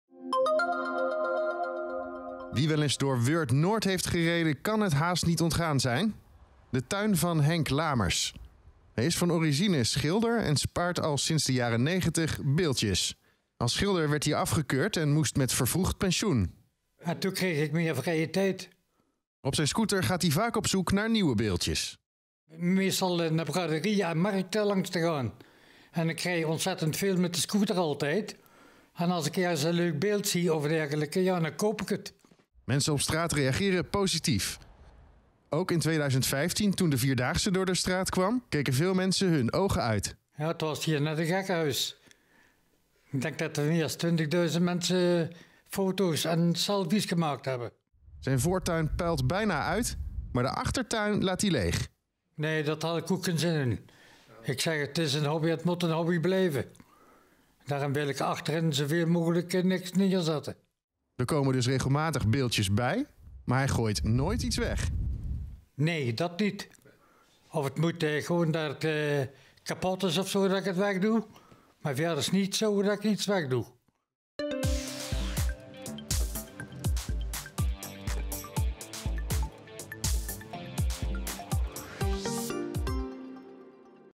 Weurtse beeldjesverzamelaar doet er nóóit één weg
WEURT - Allemaal verschillende beelden kleuren de voortuin van Henk Lamers. Vroeger een schilder, nu een fervent verzamelaar. Met meer dan honderd verschillende soorten beeldjes is Henk zijn tuin een opvallende verschijning in het straatbeeld van Weurt-Noord. "Als ik ergens een leuk beeld zie, dan koop ik het.”